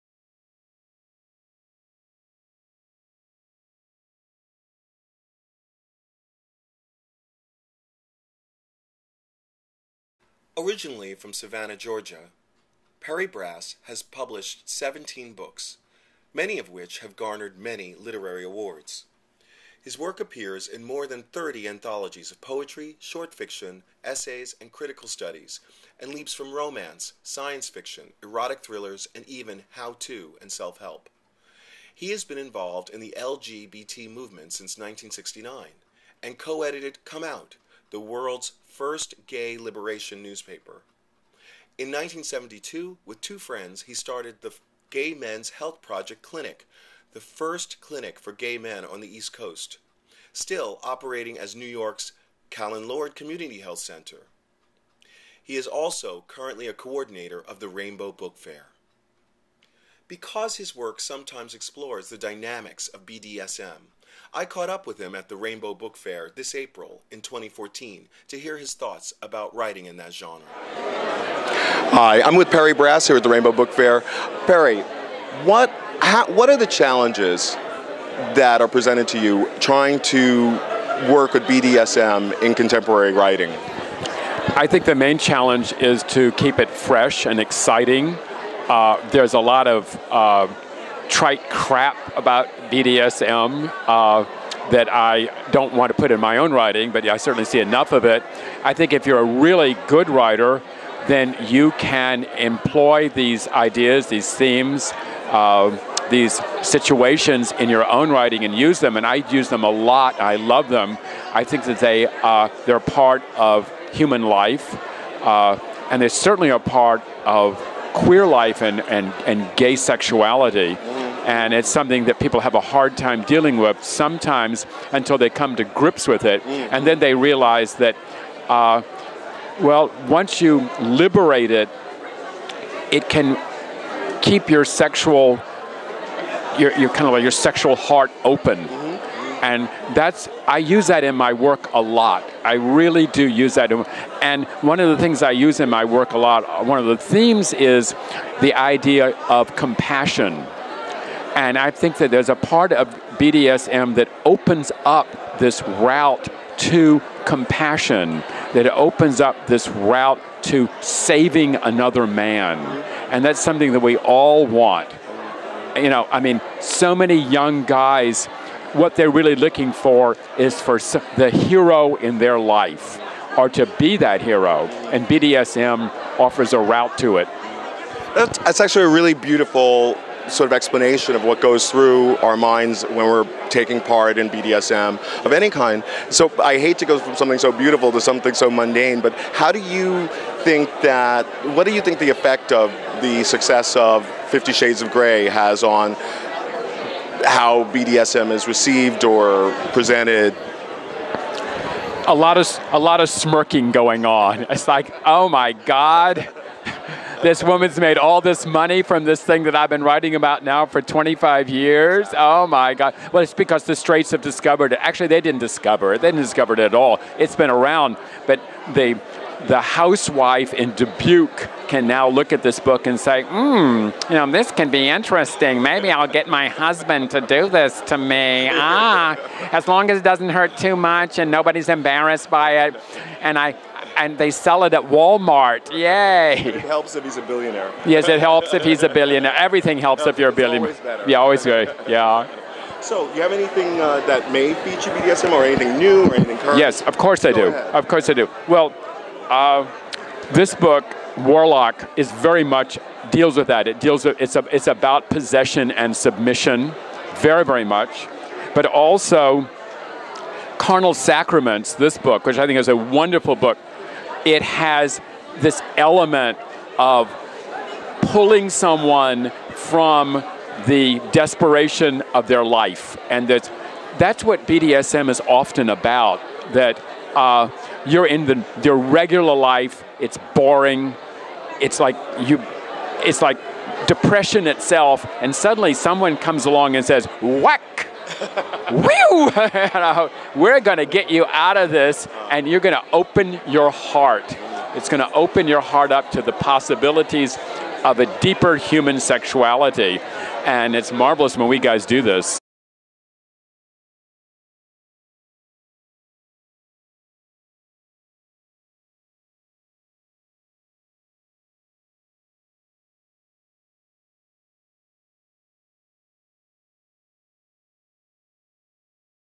Perry Brass at RBF 2014
Weeks before the release of 50 Shades of Grey, Darrell asks seasoned lgbt bdsm writers about the state of the genre.